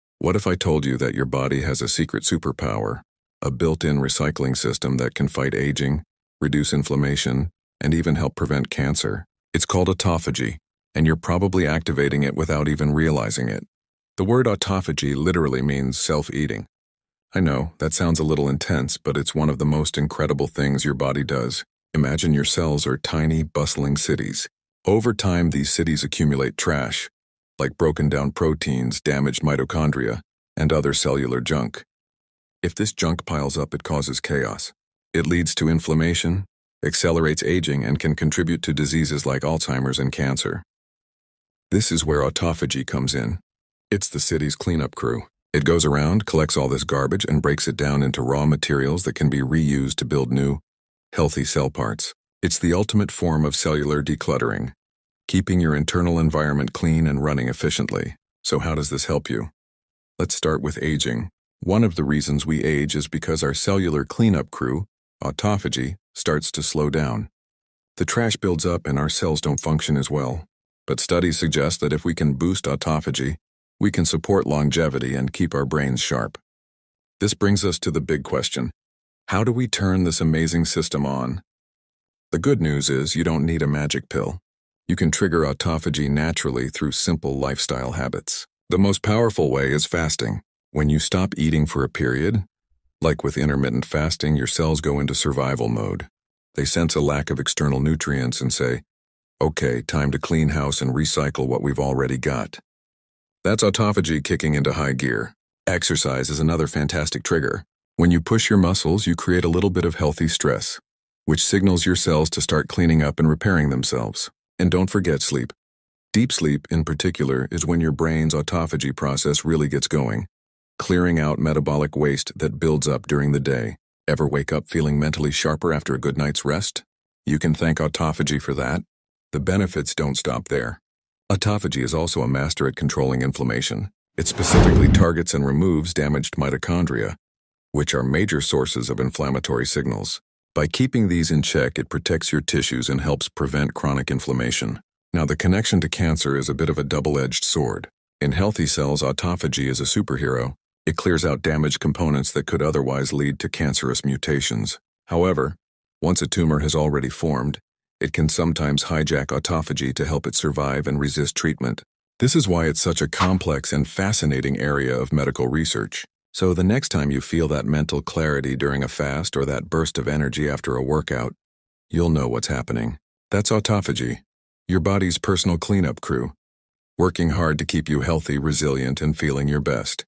Autophagy: Your Body’s Hidden Superpower Against Aging, Cancer & Inflammation
What if your body had a built-in detox system that could fight aging, cancer, and inflammation—all without a single supplement? Meet autophagy, your cells’ natural cleanup crew.

In this video, we break down the science of autophagy in a way that’s easy to understand and surprisingly fun. You’ll learn:

🔹 What autophagy is and how it works
🔹 How it helps slow aging and boost longevity
🔹 Its role in preventing cancer and chronic inflammation
🔹 Simple ways to activate autophagy—like fasting, exercise, and sleep
🔹 Real-life examples of how you’ve already felt its effects

Whether you're into biohacking, healthy aging, or just curious about how your body heals itself, this is your crash course in one of biology’s most powerful repair tools.

Autophagy